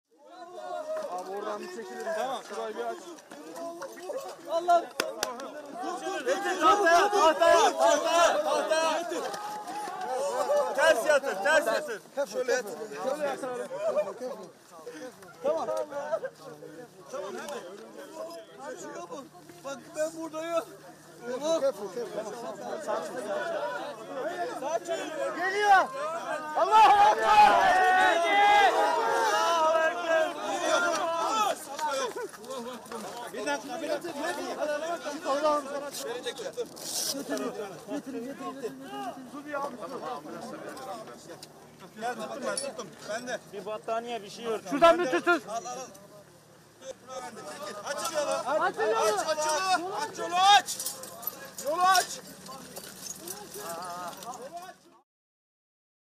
Erdbeben: Wundersame Rettung nach mehr als 150 Stunden | AFP
Wundersame Rettung im türkischen Erdbebengebiet: Mehr als 150 Stunden nach dem verheerenden Erdbeben können Rettungskräfte aus El Salvador und der Türkei einen kleinen Jungen und eine junge Frau lebend aus den Trümmern bergen.